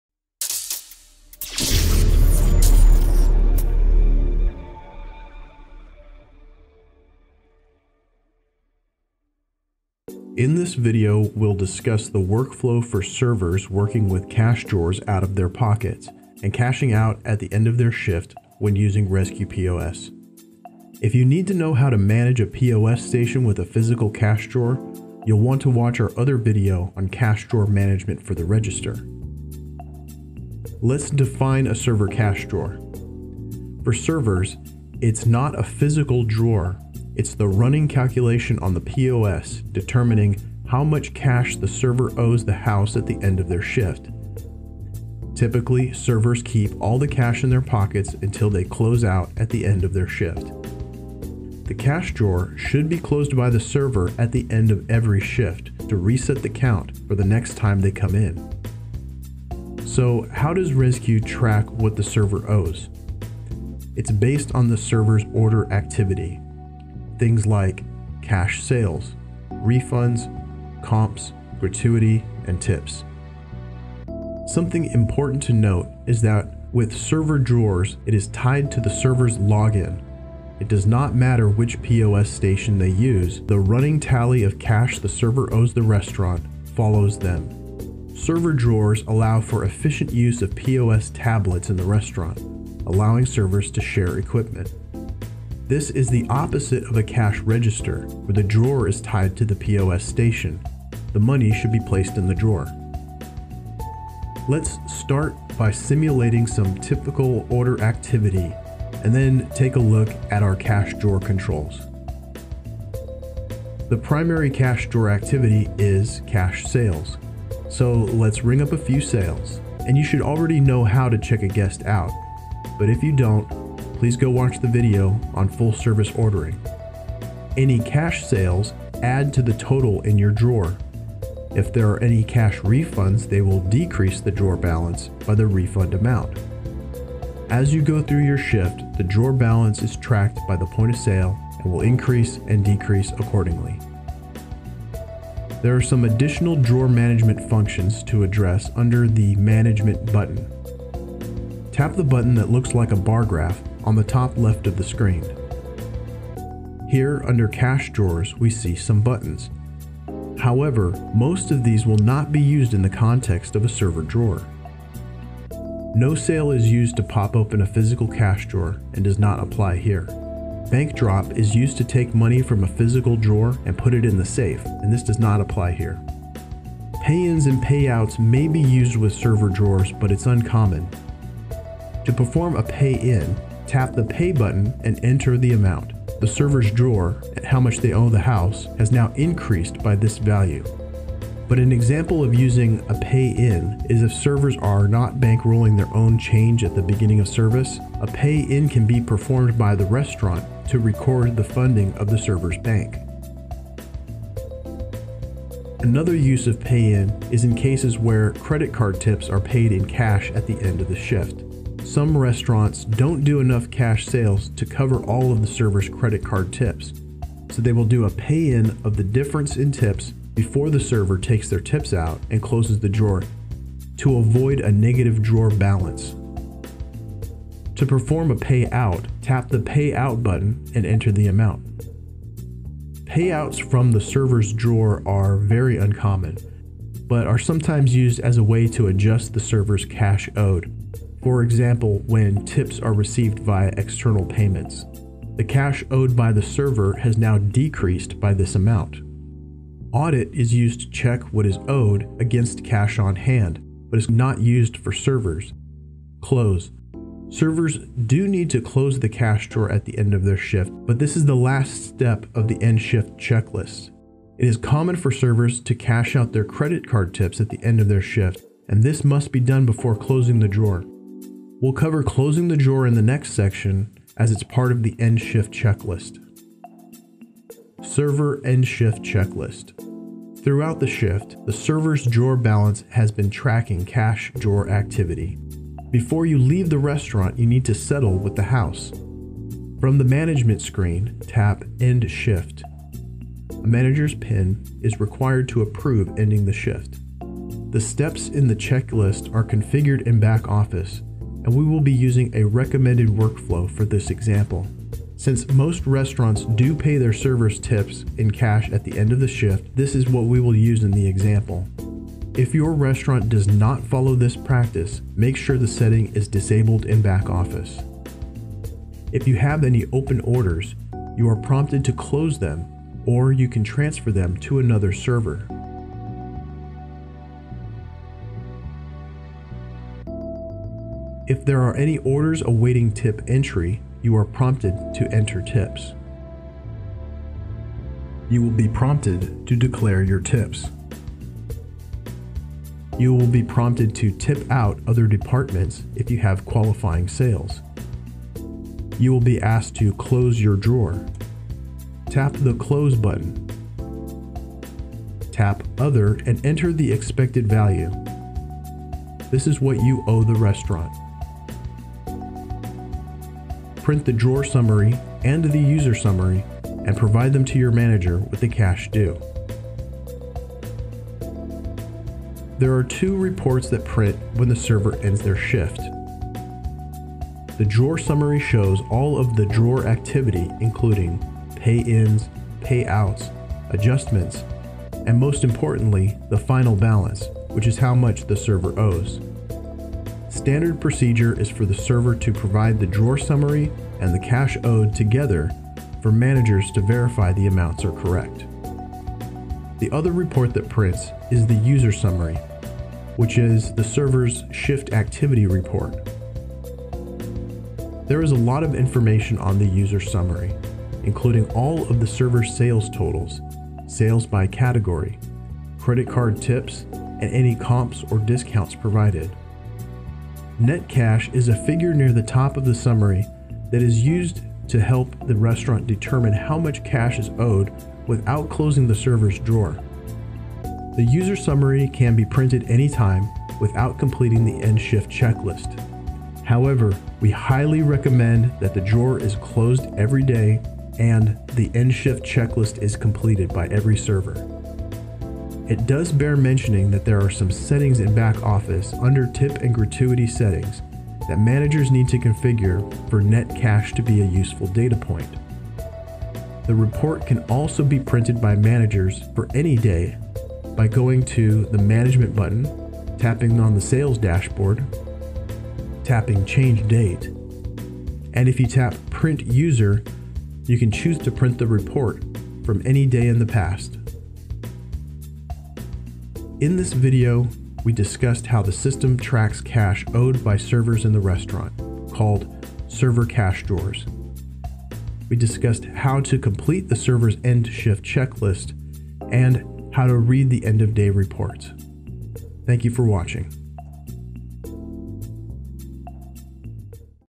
Server Drawers & End Shift Checklist | Rezku POS Training
This training video is for users of Rezku's point of sale app. This video is designed to help new users understand how servers in the restaurant manage cash and settle with the restaurant at the end of the day.

Video Chapters:
0:00 Intro
0:30 Server Cash Drawer Defined
1:03 Server Drawer Activity Examples
2:25 Drawer Management Buttons
4:37 End Shift Checklist
5:48 Closing Server Cash Drawers
6:11 End Shift Reports
7:36 Reprinting User Summary Reports
7:55 Summary